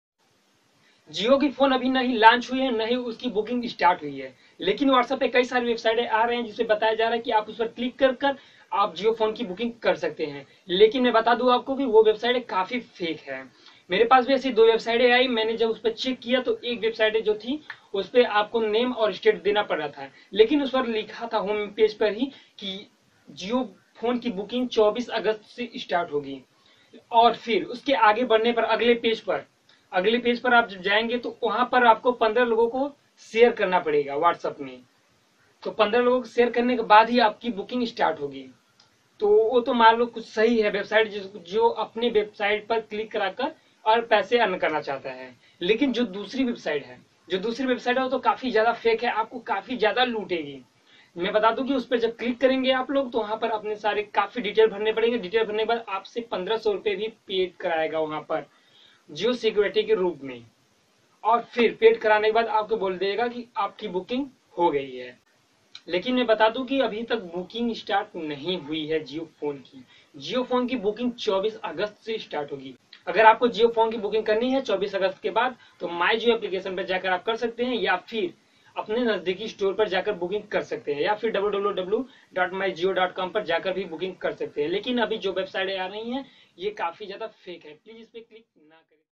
Book online jio phone , Don't book form these link
In this video, You will know jio Phone booking fake website that with you.

If you have any problem  you can comment.
And also can follow on